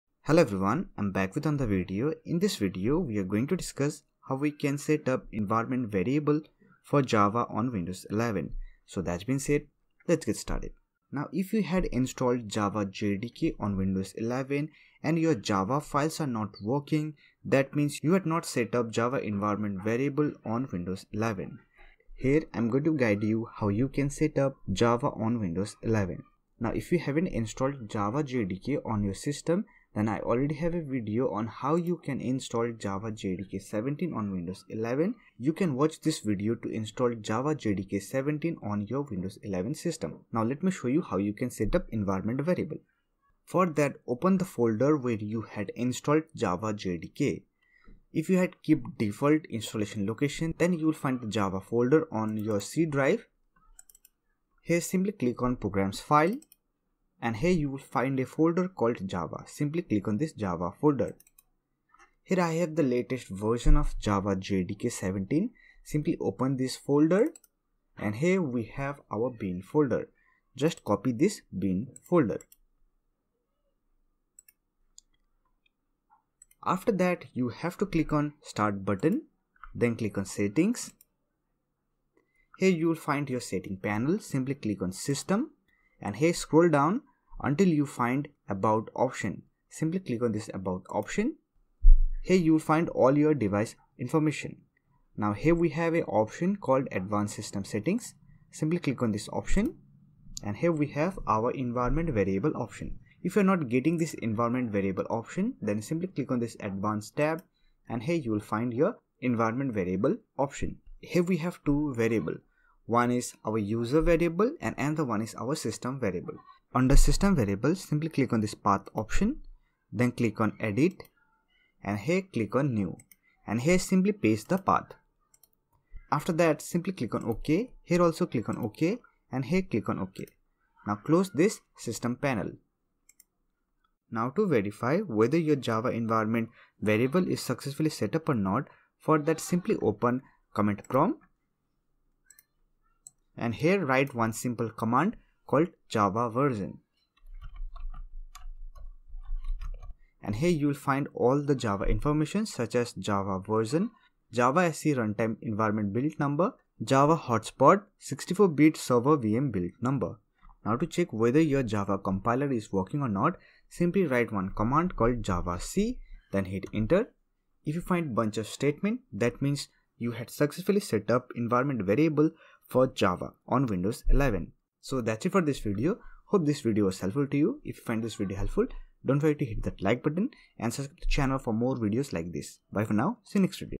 How to Setup Environment Variables for Java in Windows 11
In this video, we are going to learn How to Setup Environment Variables for Java on Windows 11. If you have installed Java JDK on Windows 11 and the Java environment variable is not set up java files and programs will not work.

Video Highlights
00:00 Introduction
00:11 Setup Environment Variable for Java
02:14 Verify Java Installation on Windows 11
02:55 Conclusion